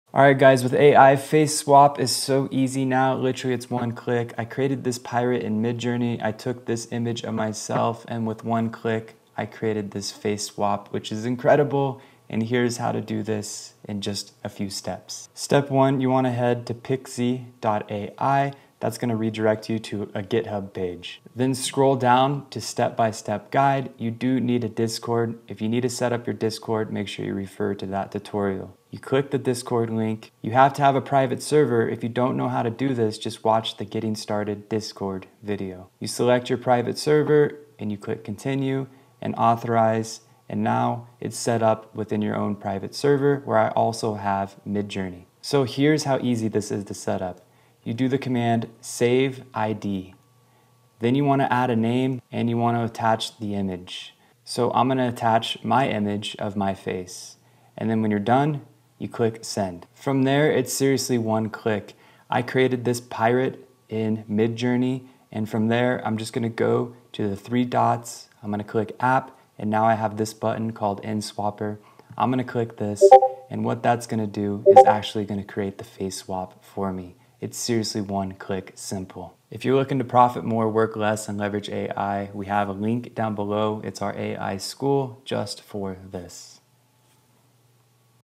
Using Midjourney to Keep My Face
This is how I'm using midjourney to keep my face in generatated Ai images. This is the best way to keep my face in Midjourney I have found and it works great!